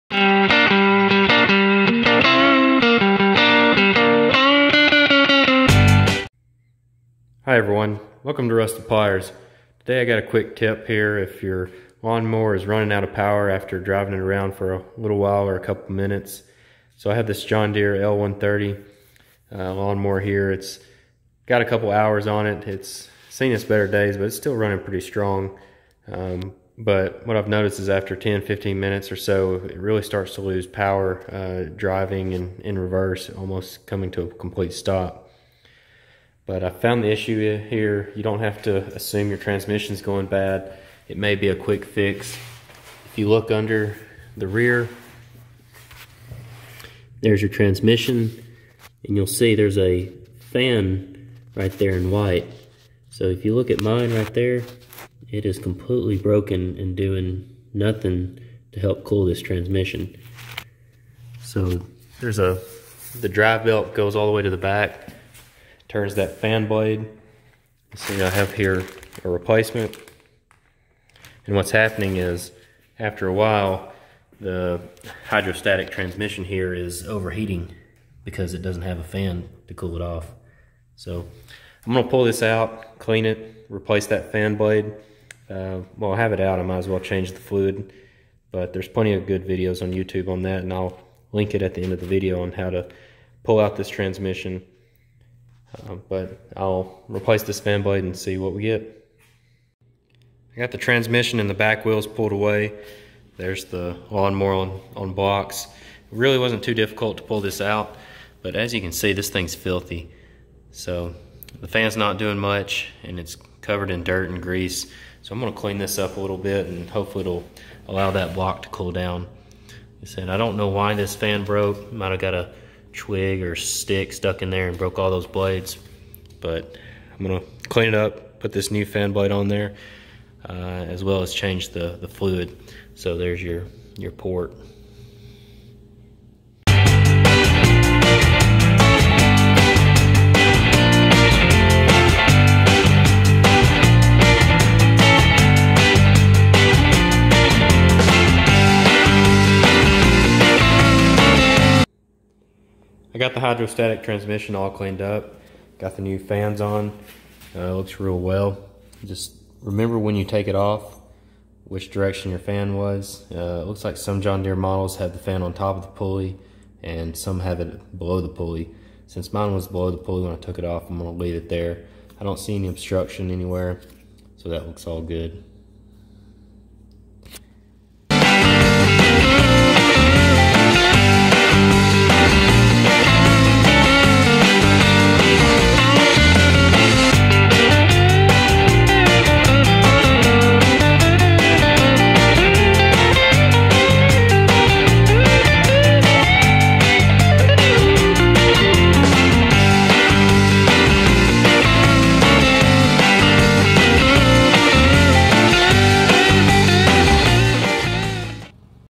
How to Fix John Deere Lawn Mower Losing Power, Slows Down
My John Deere L130 Lawn Mower was slowing down after using it for awhile. It would get so slow it would hardly drive or move in forward or reverse. I replaced the broken fan blades and now it works great! This fix applies to all John Deere 100 series Mowers.
Fan Blade
Hydrostatic Oil